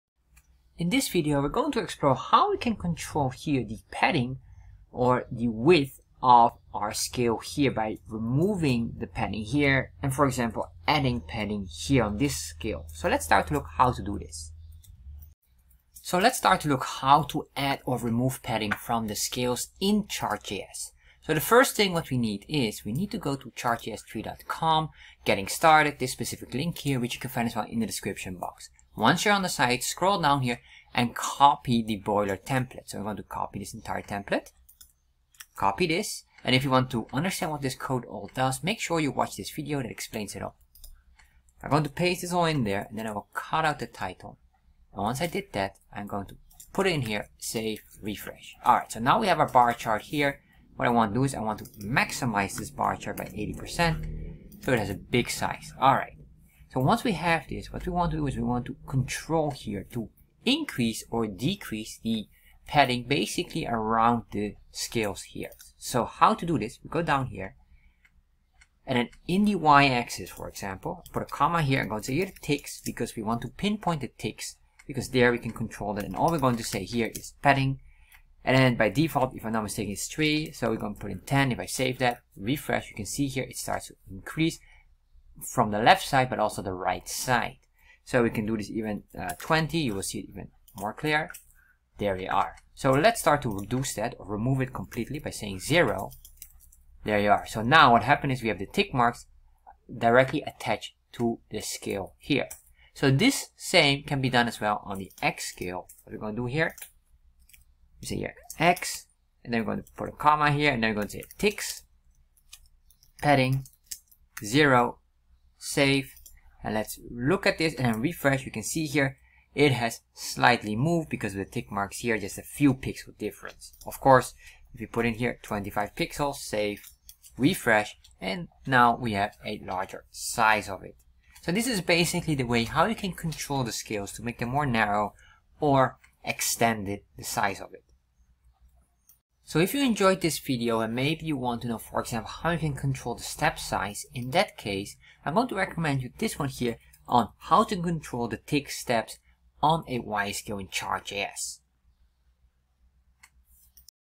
How to Add or Remove Padding From the Scales in Chart JS
In this video we will explore how to add or remove padding from the scales in chart js. To control the scales size by stretching or narrowing the scale with padding. Luckily this is a quite straightforward process. Which comes with Chart.js out of the box.

▬ Materials/References  ▬▬▬▬▬▬▬▬▬▬

Chart JS Dashboard Series:
👍 Most Liked Video Series:

▬ About Us  ▬▬▬▬▬▬▬▬▬▬▬▬